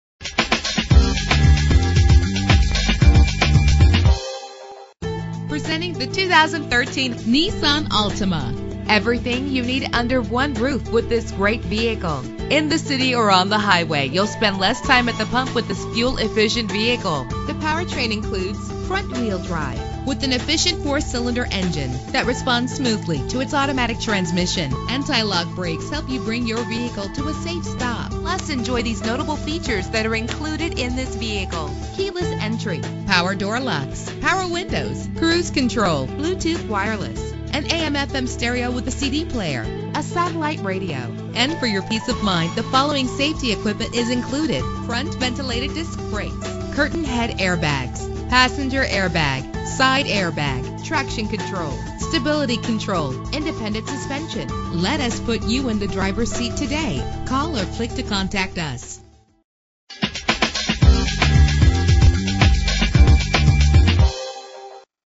2013 Nissan Altima - Daphne AL
Year: 2013
Make: Nissan
Model: Altima
Trim: 2.5 SV
Engine: 2.5 liter inline 4 cylinder 16 valve
Transmission: Automatic CVT
Color: Saharan Stone Metallic
Mileage: 10
Address:
27161 Hwy. 98